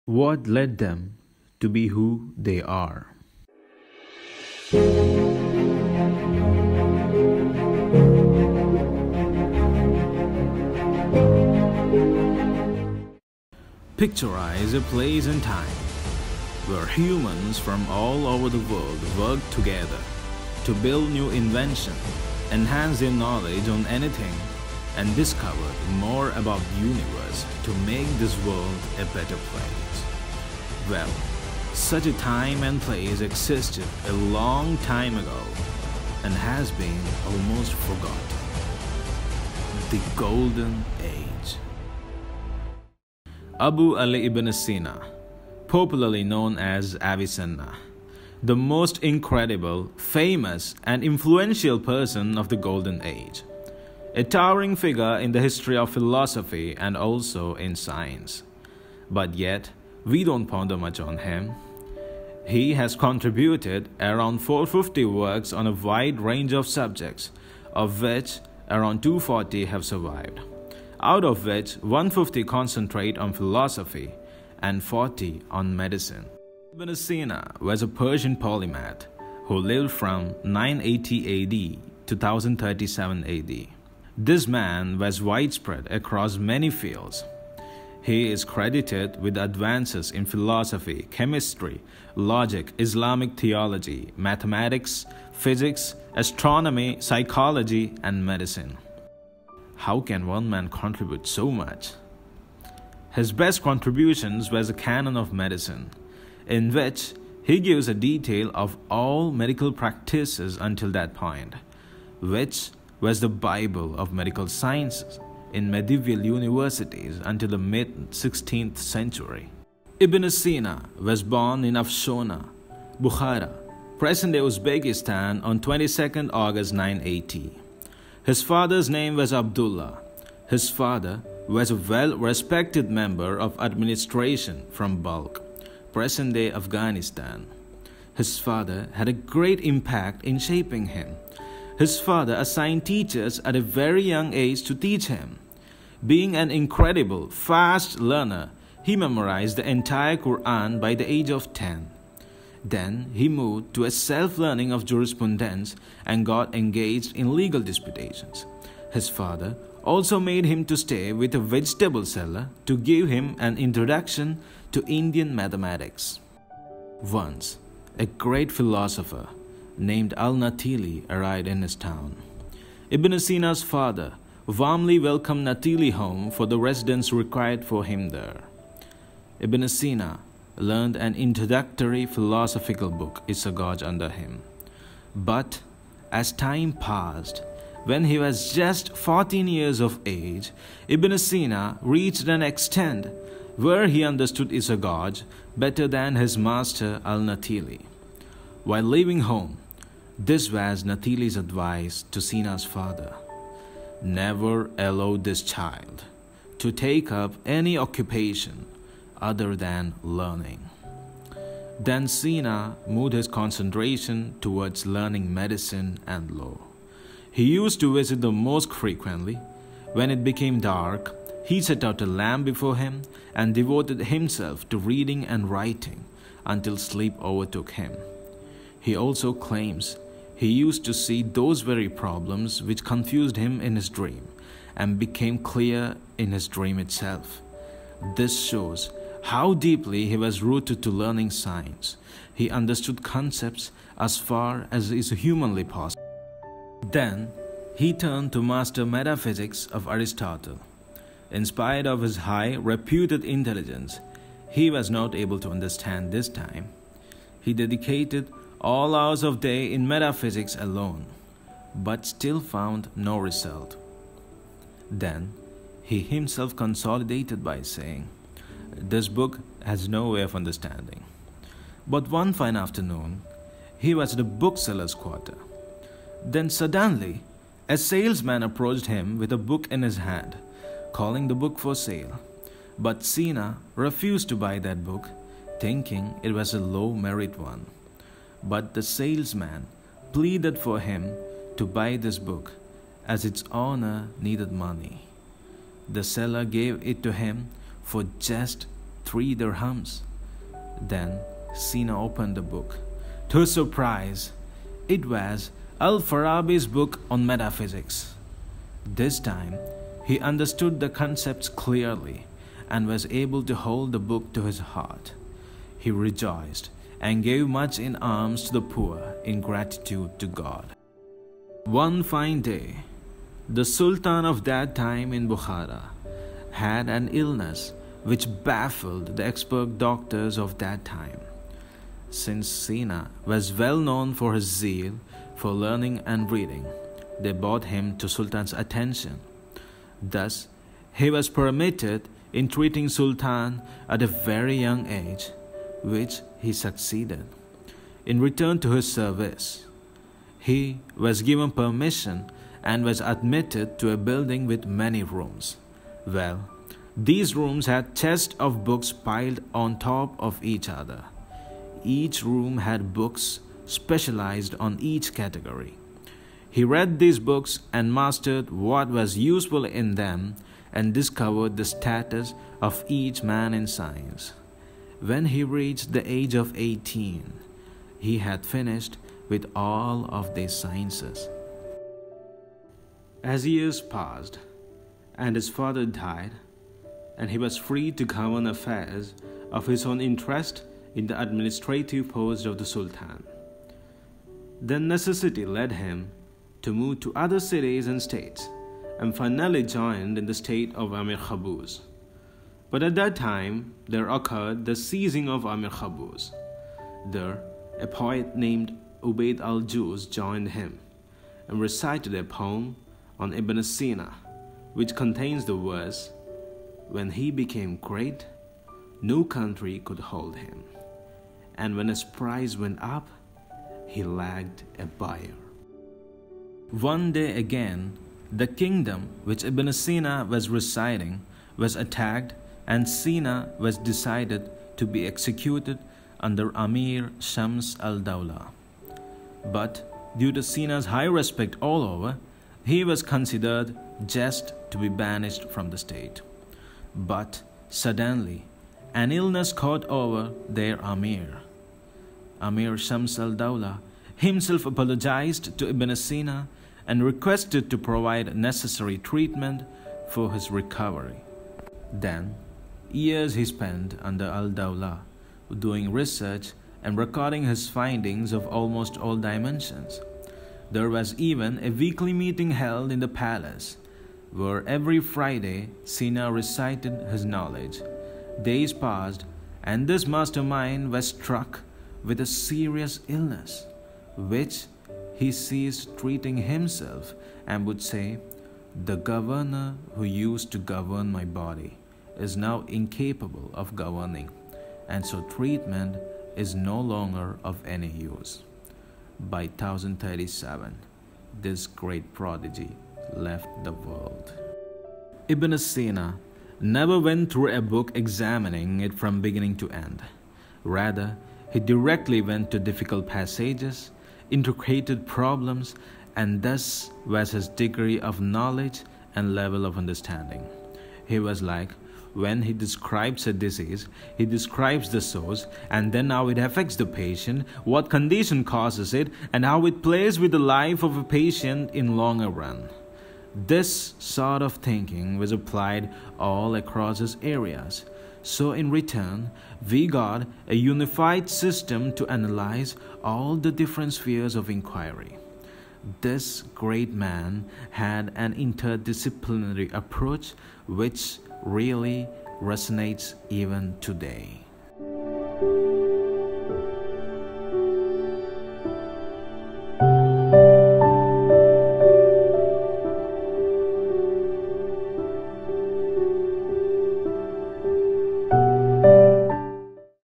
What led them to be who they are? - Ibn Sina
Known in the West as Avicenna, With expertise in many fields, from medicine and astronomy to mathematics and theology. Ibn Sina was a Persian polymath and the father of early modern medicine. As a physician, he dominated thought during the period from the late twelfth to the late seventeenth century, during which time his Canon of Medicine was the single most important medical text in all of Europe.

Study and narration by-
Akhin E M
AIIMS Mangalagiri